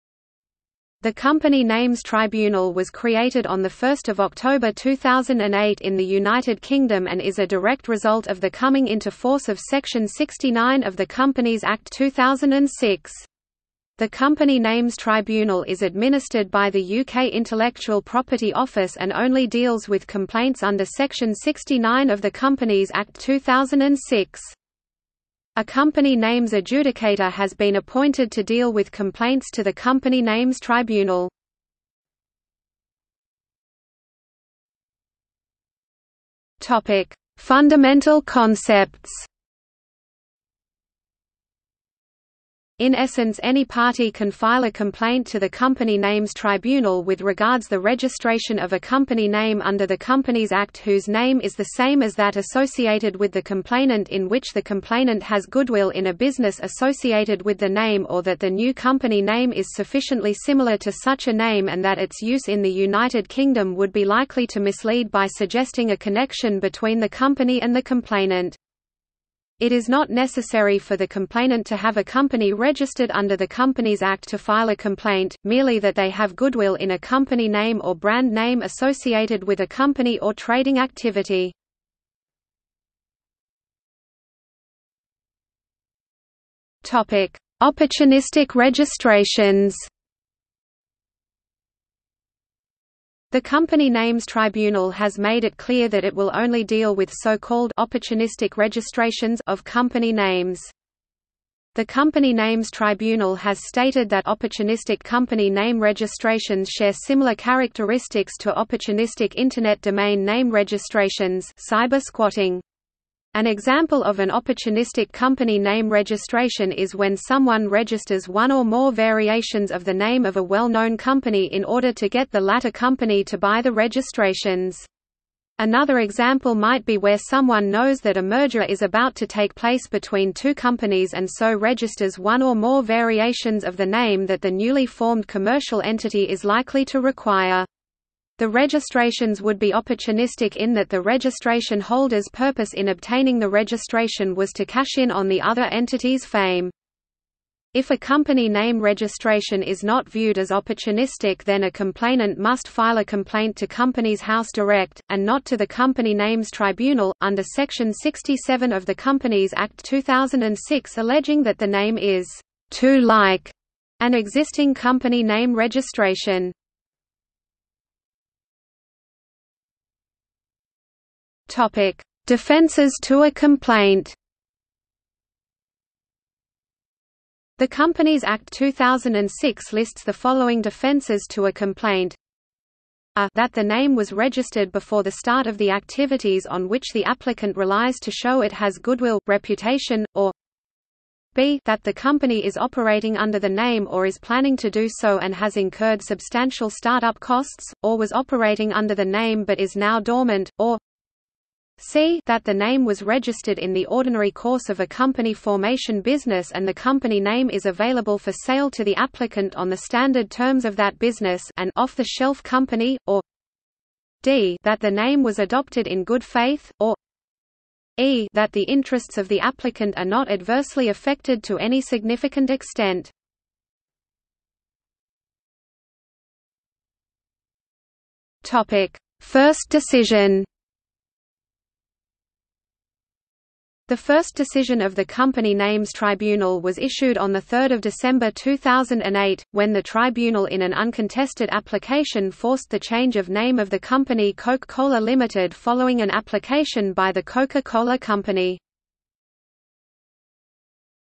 Company Names Tribunal | Wikipedia audio article
00:00:33 1 Fundamental concepts
00:01:23 2 Opportunistic registrations
00:02:48 3 Defences to a complaint
00:03:44 4 First decision

- Socrates

SUMMARY
The Company Names Tribunal was created on 1 October 2008 in the United Kingdom and is a direct result of the coming into force of Section 69 of the Companies Act 2006. The Company Names Tribunal is administered by the UK Intellectual Property Office and only deals with complaints under Section 69 of the Companies Act 2006.
A Company Names Adjudicator has been appointed to deal with complaints to the Company Names Tribunal.